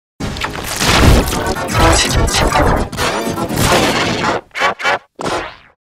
Klasky Csupo In G Major 101
There are the new G majors coming up, they go from G major 101 to G major 121.